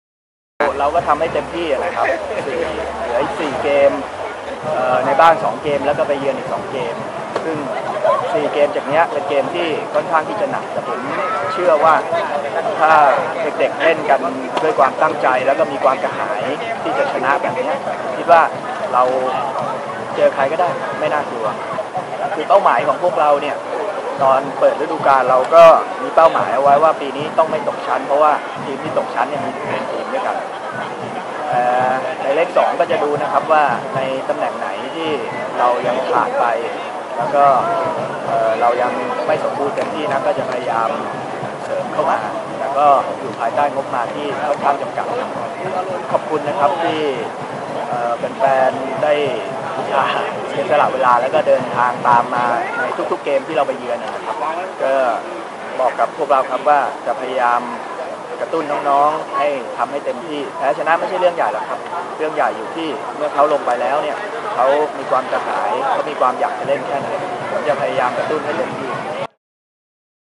บิ๊กแบดเชื่อหากลูกทีมเล่นด้วยความกระหาย อ่างทองเจอใครก็ได้
"บิ๊กแบด" ภราดร ปริศนานันทกุล ผจก.สโมสรอ่างทอง เอฟซี แสดงความเชื่อมั่นหากลูกทีมเล่นกันด้วยความกระหายชัยชนะ ไม่ว่าเจอกับสโมสรไหนก็สามารถสู้ได้ทุกทีม